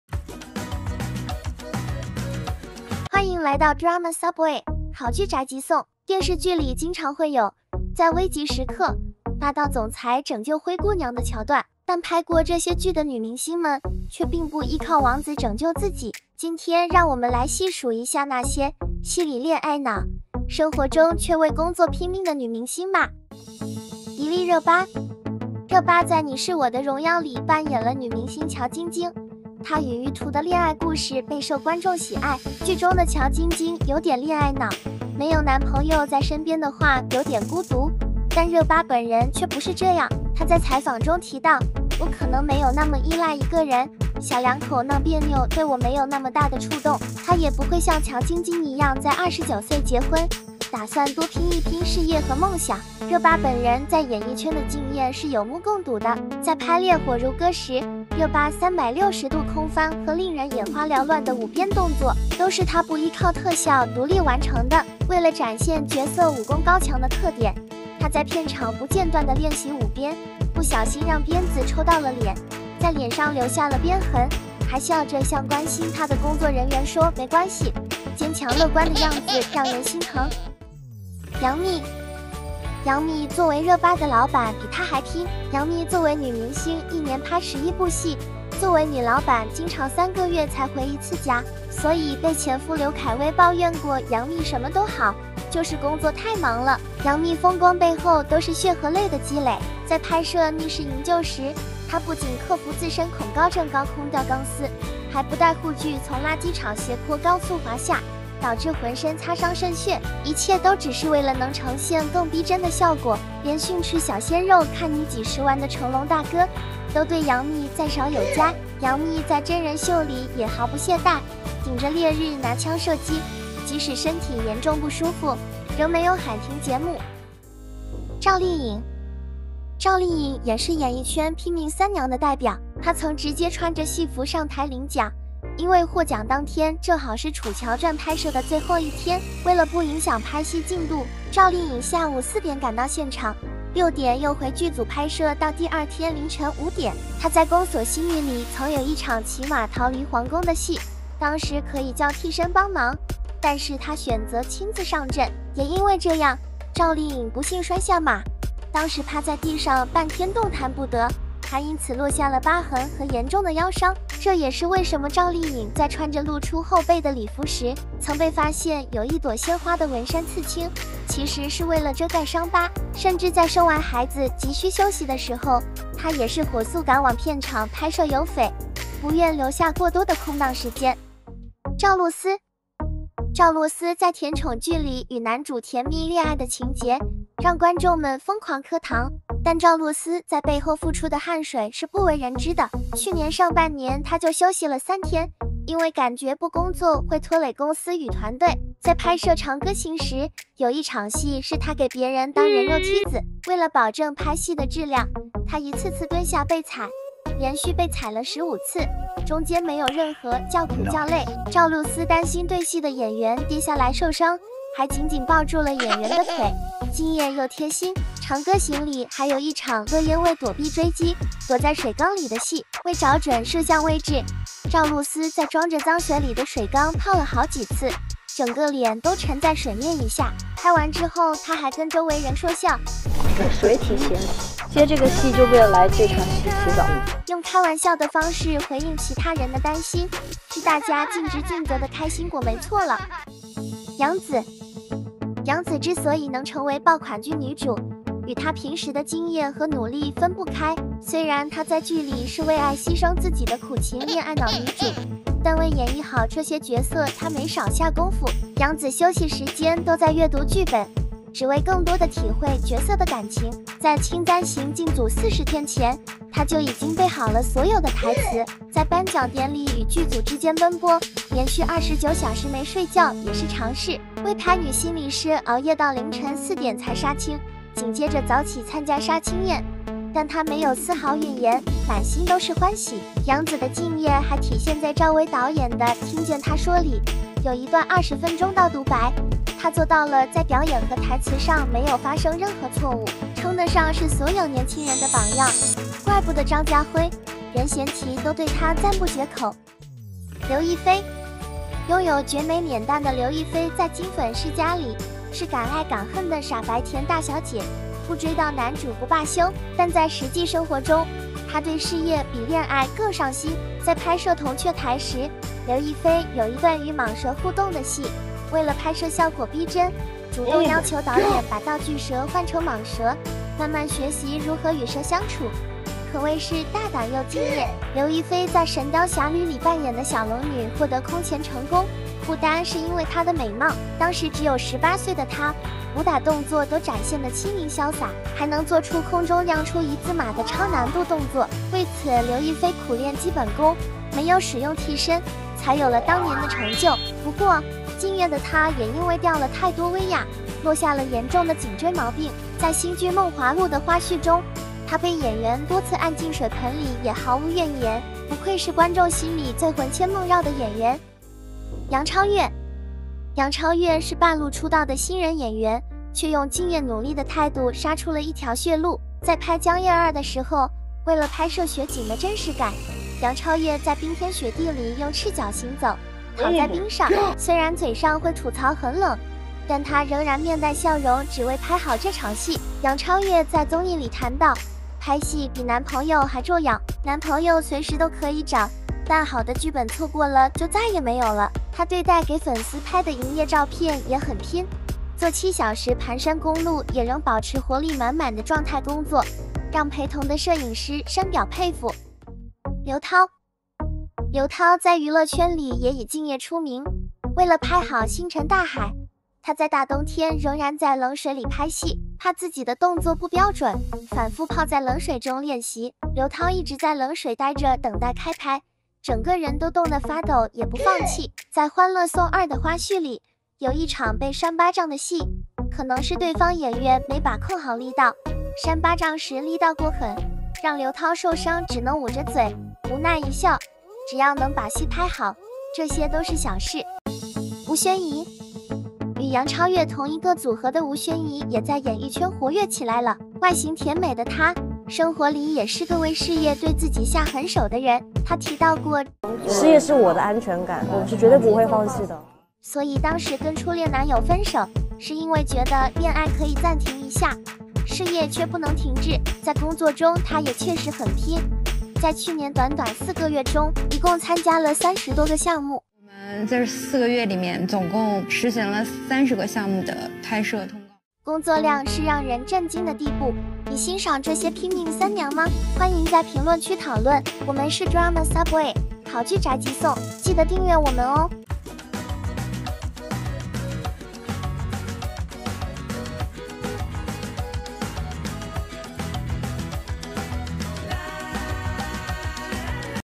剧中恋爱脑实际女强人？赵露思热巴都是娱乐圈的隐形高手|随意扒一扒|Drama Suwbay
剧中恋爱脑实际女强人？赵露思热巴都是娱乐圈的隐形高手